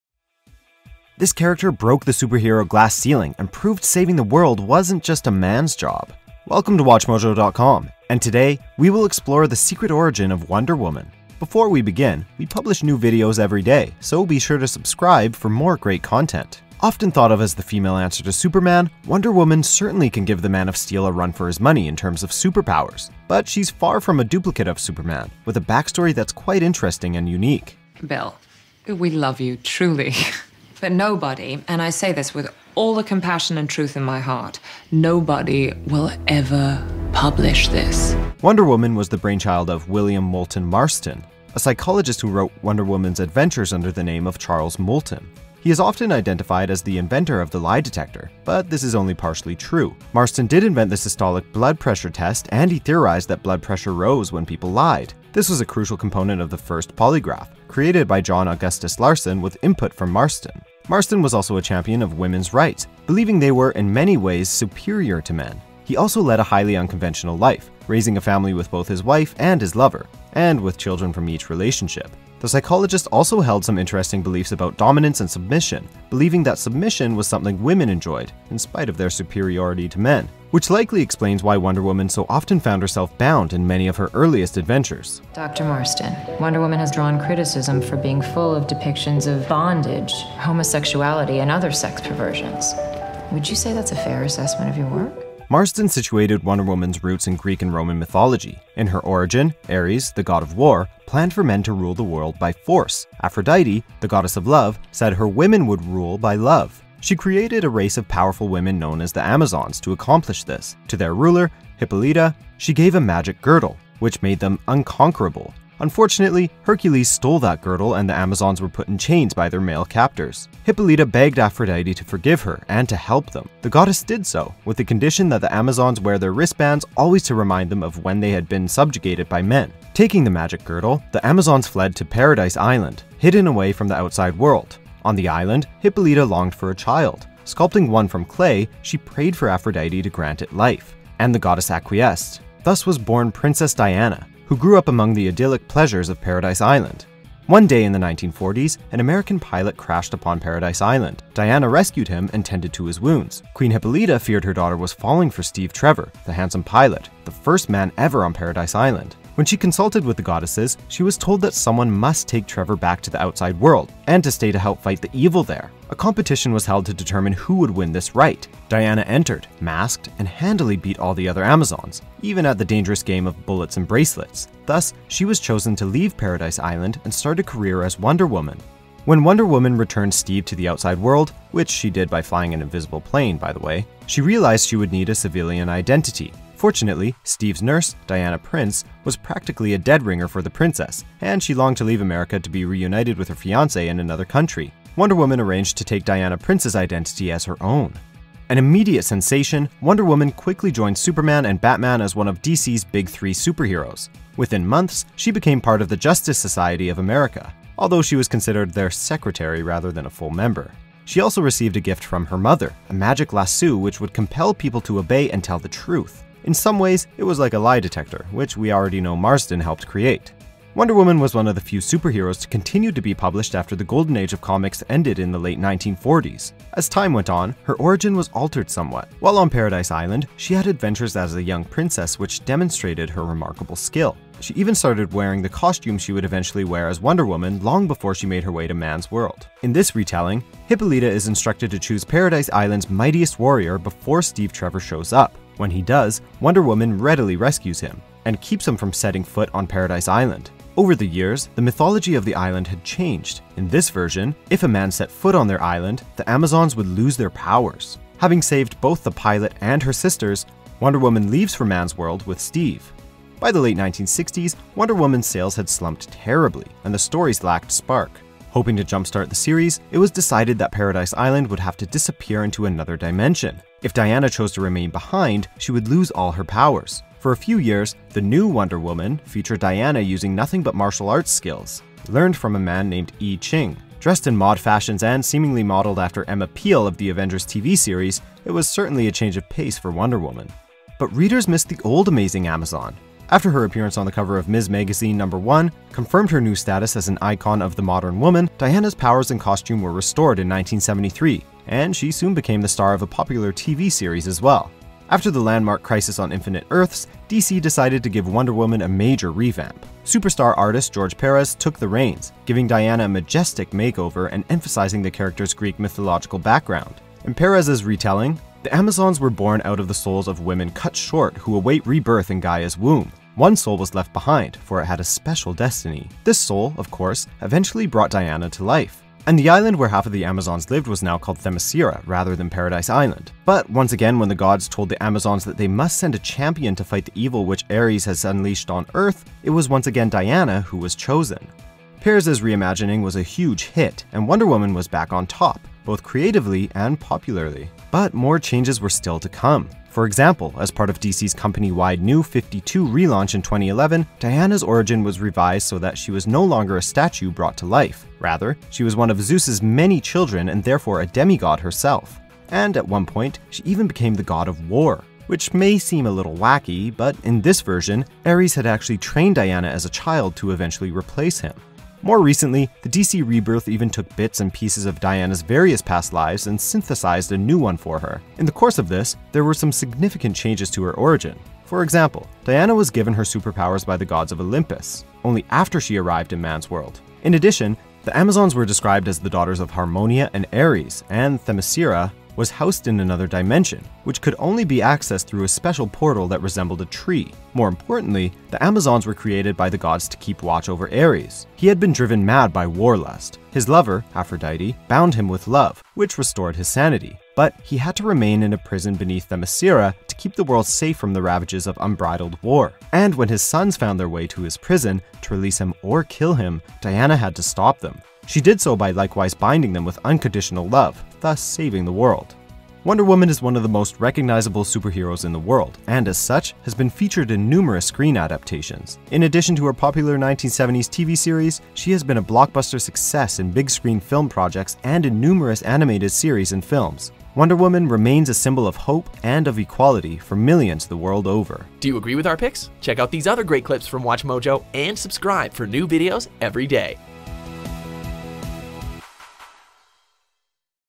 Secret Origins: Wonder Woman
Wonder Woman is one of the most iconic superheroes of all time. She also happens to have one of the most interesting backstories of any character in comic book history, both on the page and behind the scene. In this installment of Secret Origins, we'll be taking a look at Diana's storied past, and the people who helped to turn her into the icon she's become.